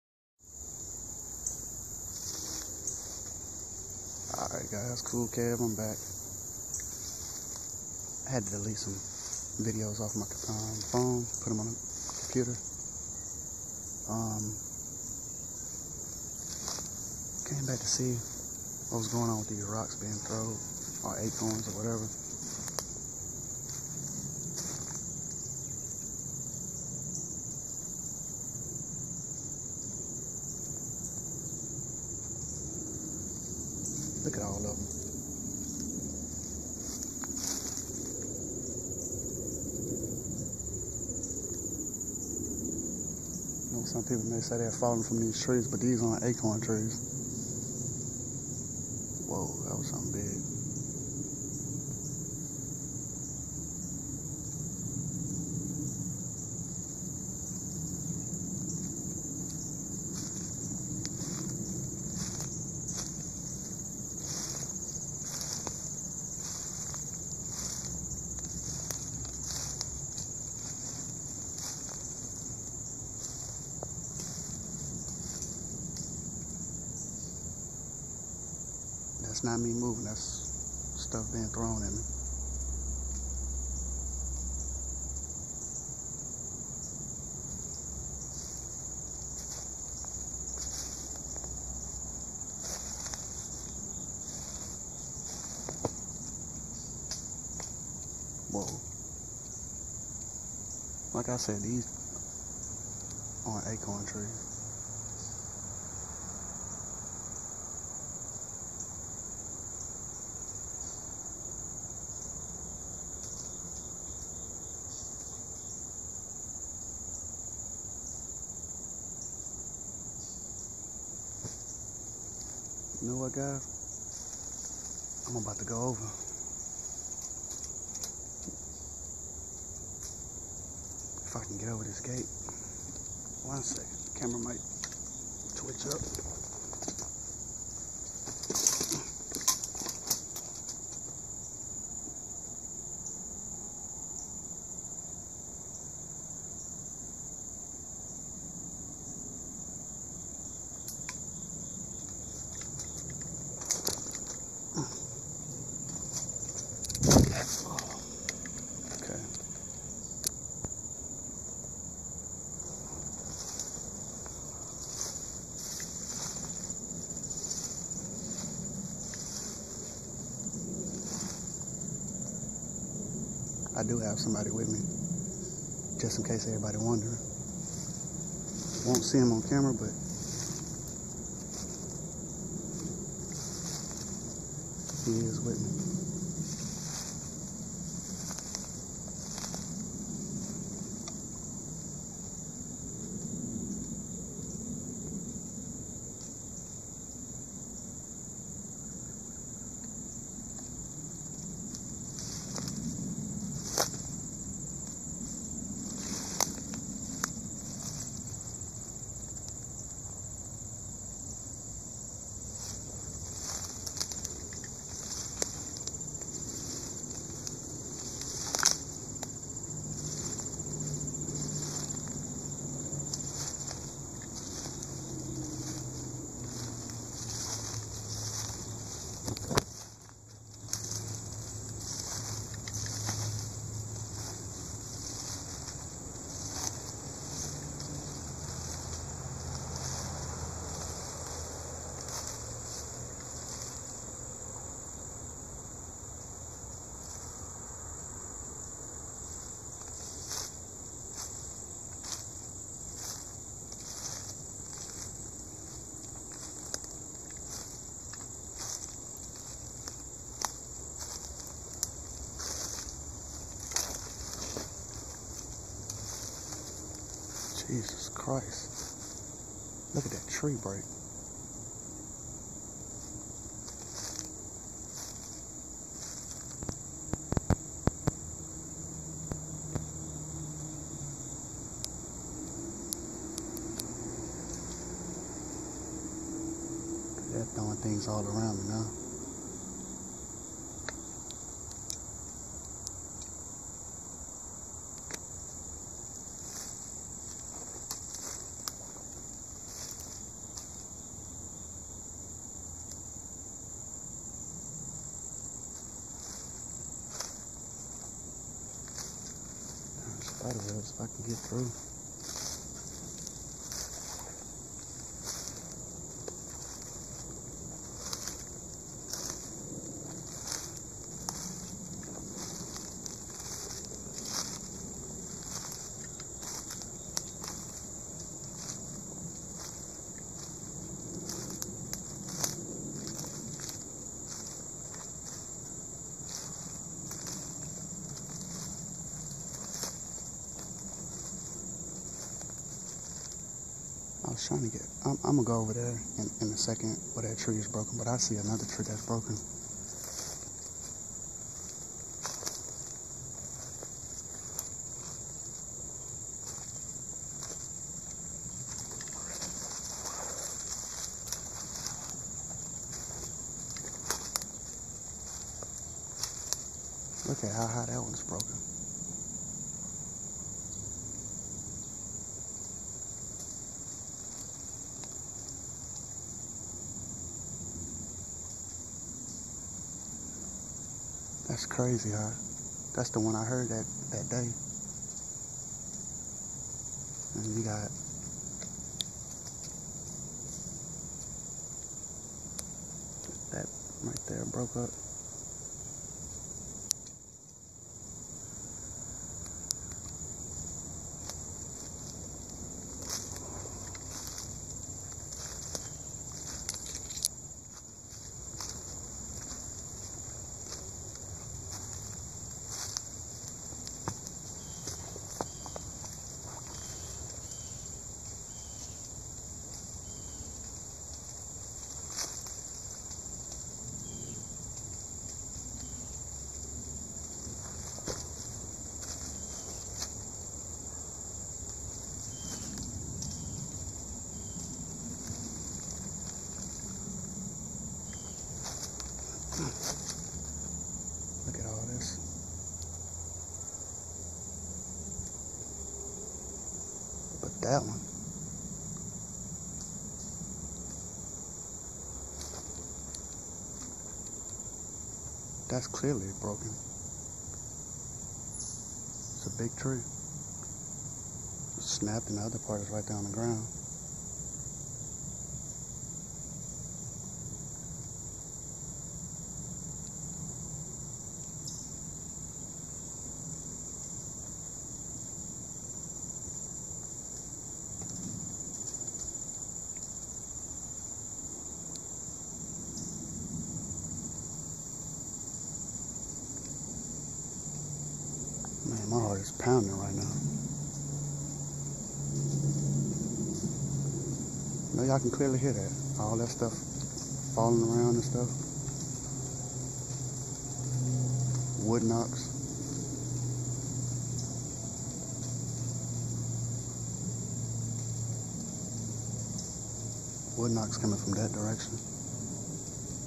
SASQUATCH??? Over the gate again part. 1
Guys I just went over the fence in my backyard cause I saw some really big trees broken from the road that runs through my area